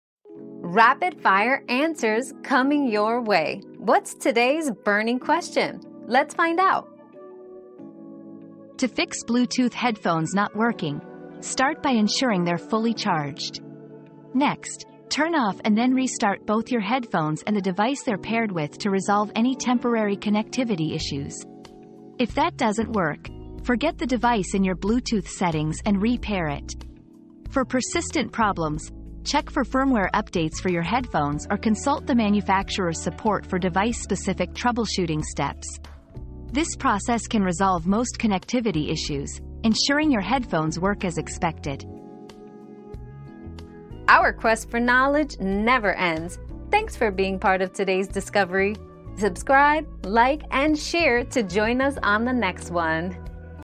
How do I fix my Bluetooth headphones not working?
Troubleshooting Bluetooth Headphones Not Working 👉 Fix Bluetooth Headphones 👉 Learn how to troubleshoot and fix Bluetooth headphones not working by checking battery levels, restarting devices, re-pairing, updating firmware, and seeking manufacturer support for connectivity issues.

Laura S. Harris (2024, March 12.) How do I fix my Bluetooth headphones not working?
    🌐 AskAbout.video